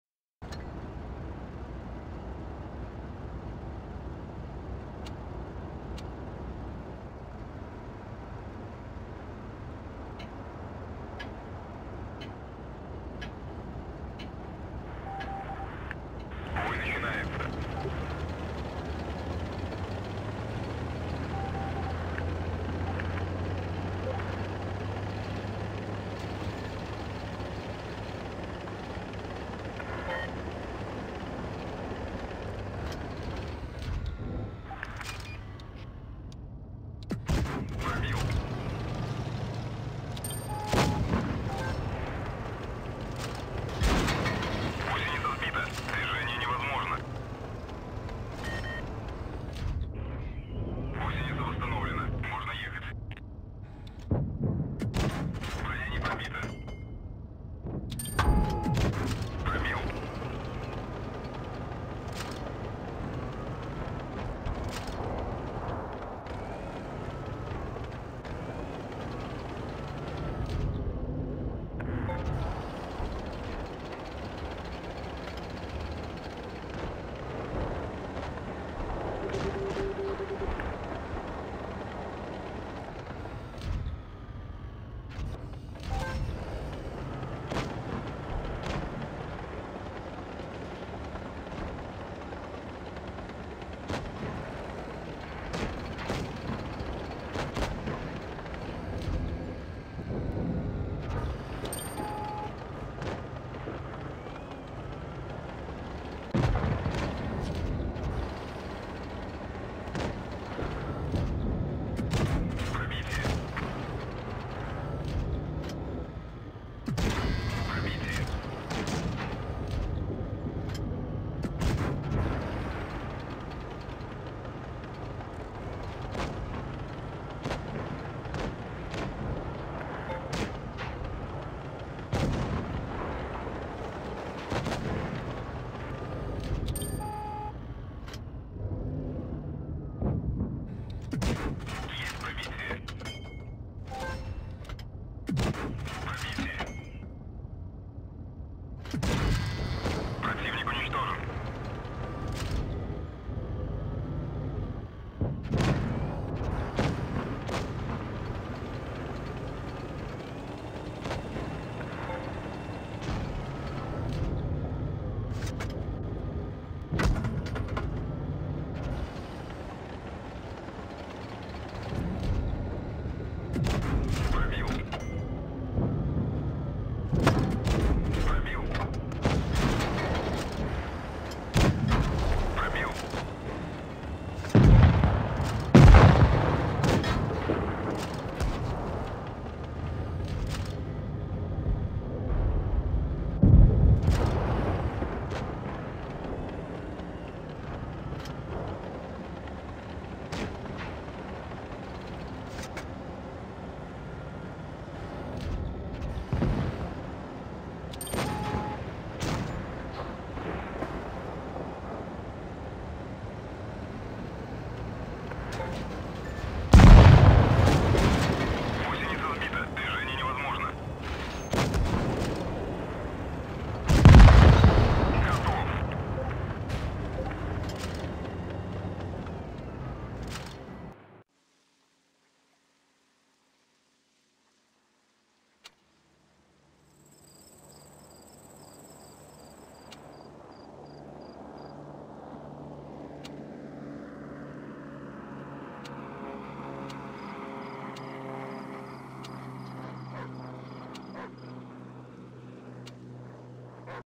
Crushing End in the K 91
Run of the mill T9 gameplay until an opportunity at the end of the game.